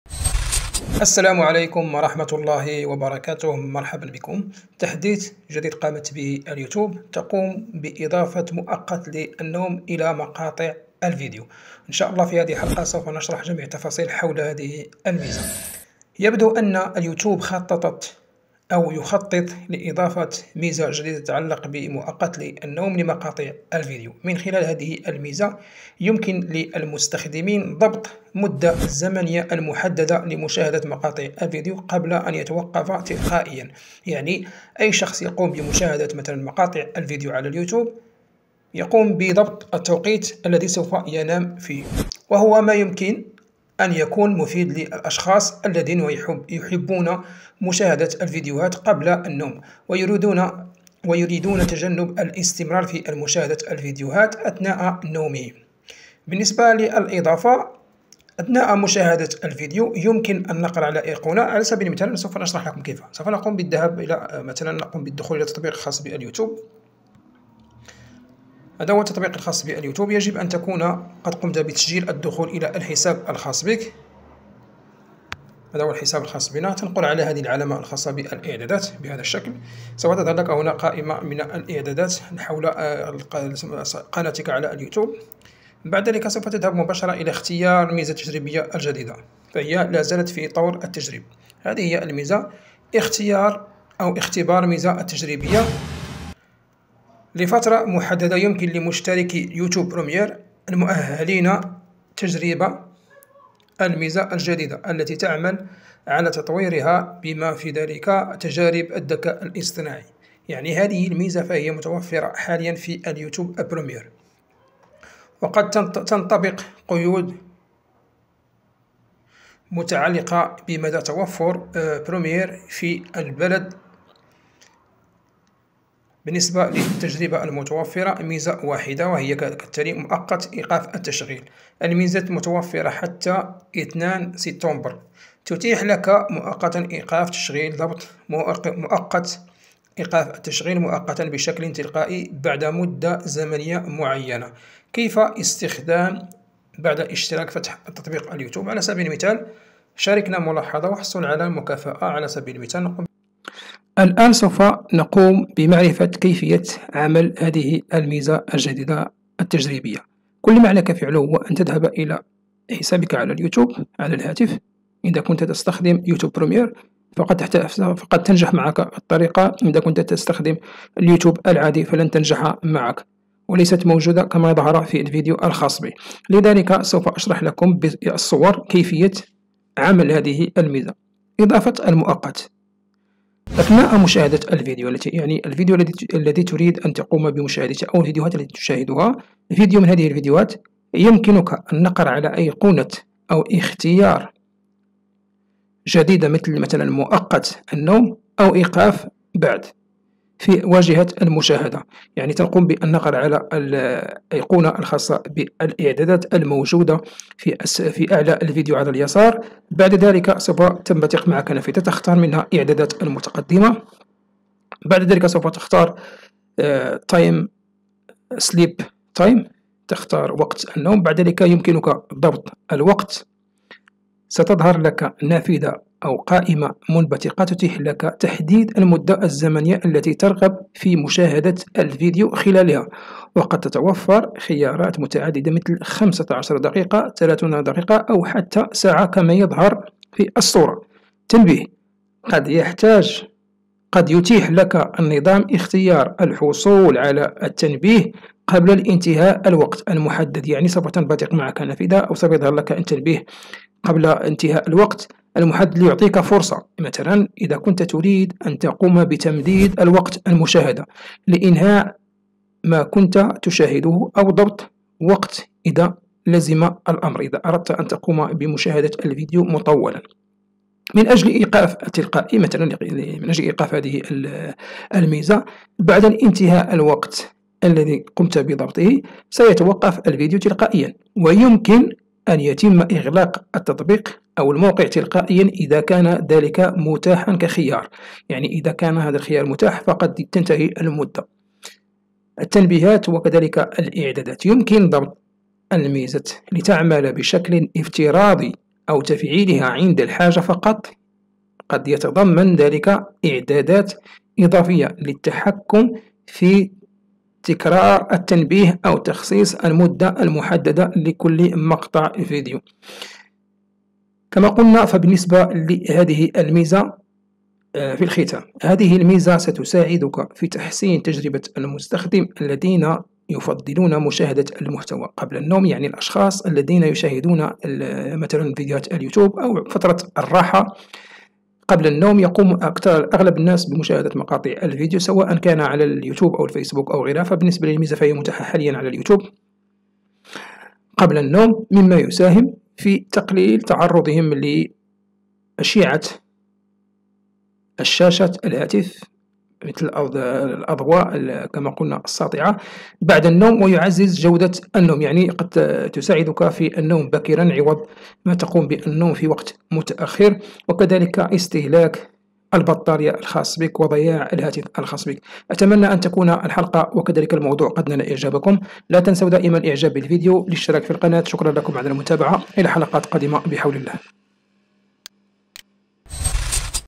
يوتيوب سيضيف مؤقت للنوم إلى مقاطع الفيديو وهذه هي الطريقة التي يعمل بها
مرحبًا بكم في قناتنا! في الفيديو اليوم، سنتحدث عن أحدث تحديث ليوتيوب الذي جلب ميزة جديدة قد تغير طريقة استخدامكم للمنصة. هل تجدون أنفسكم أحيانًا مشغولين بمشاهدة الفيديوهات لوقت متأخر؟ الآن، مع ميزة "ضبط مؤقت النوم" الجديدة، يمكنكم ضبط وقت محدد ينتهي فيه تشغيل الفيديوهات تلقائيًا. سنستعرض معًا كيف يمكنكم الاستفادة من هذه الميزة لتعزيز راحتكم وتحسين جودة نومكم، بالإضافة إلى كيفية تفعيلها والاستفادة القصوى منها. فلنبدأ!